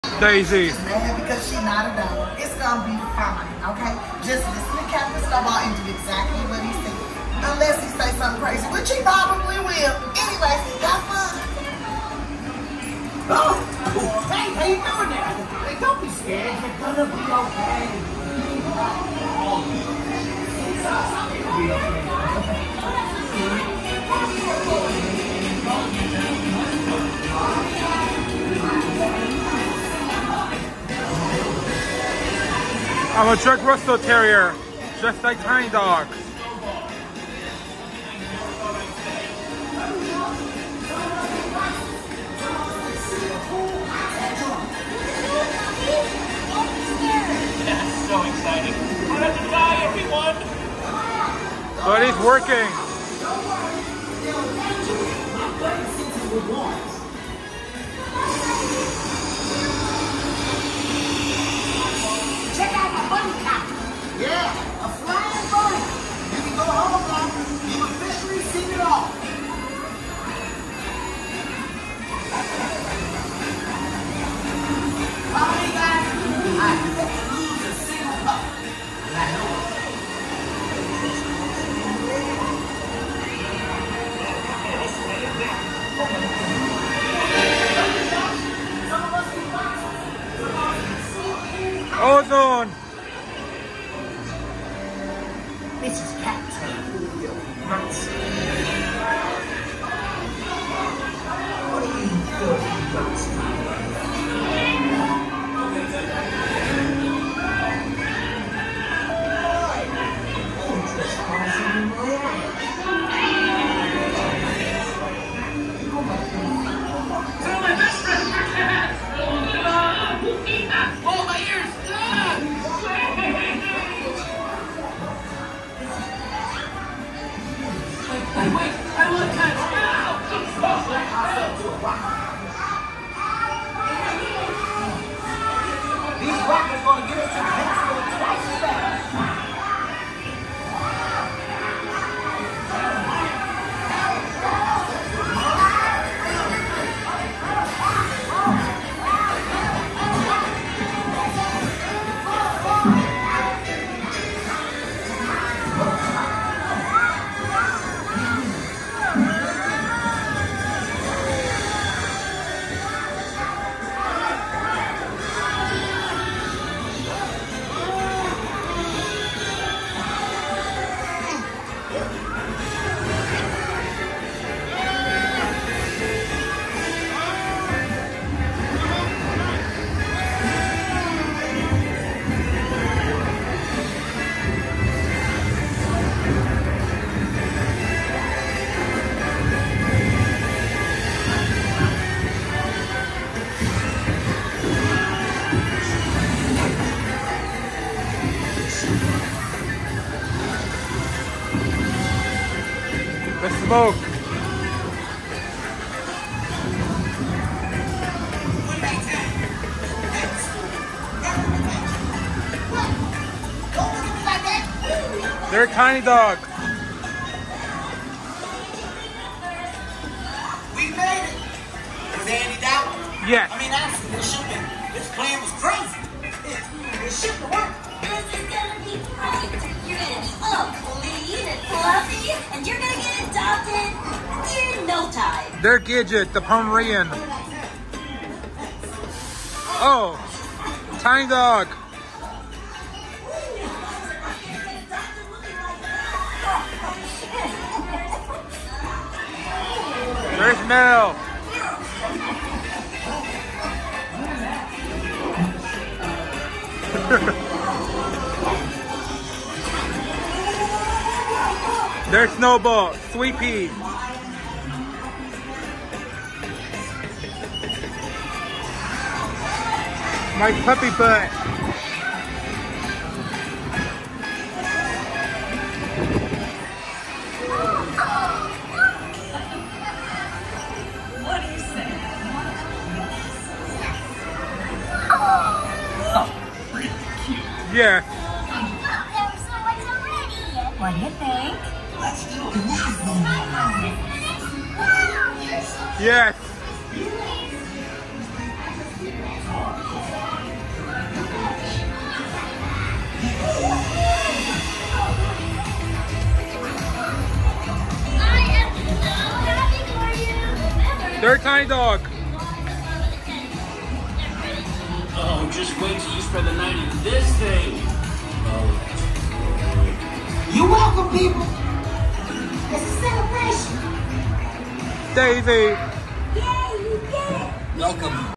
The Secret Life Of Pets: Off The Leash Ride Part 5 (2023)
This is me at Universal Studios Hollywood